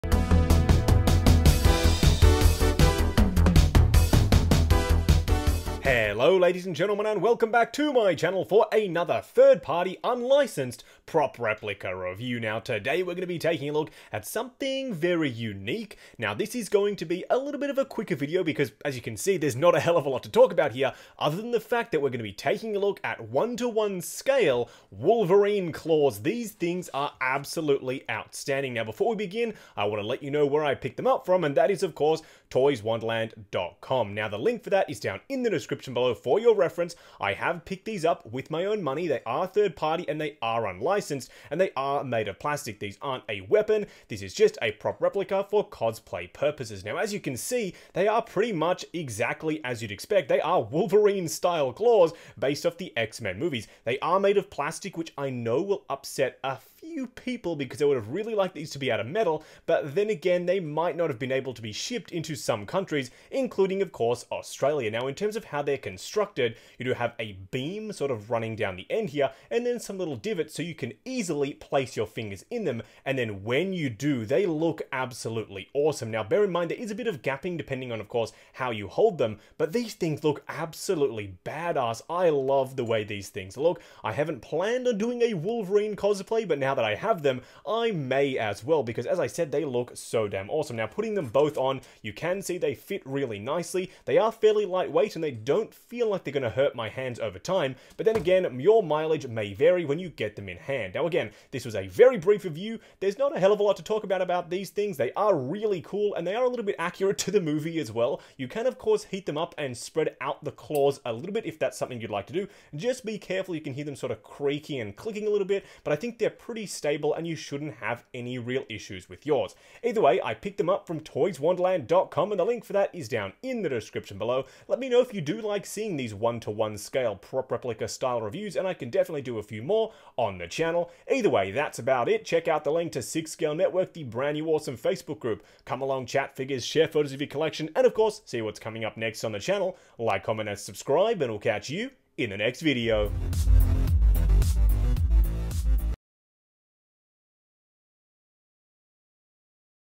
Wolverine Claws Wearable Prop Unboxing & Review - Life Size Prop Replica
Life Size Movie Prop Replica X-Men Wolverine / Logan Claws wearable Unboxing and Review!

This is a 3rd Party Unlicensed figure. This is not a counterfeit item. This review represents my opinions on the product purchased with my own funds. This is a review, not a promotional video.

Feel free to check out the social links below for hints at upcoming videos and more!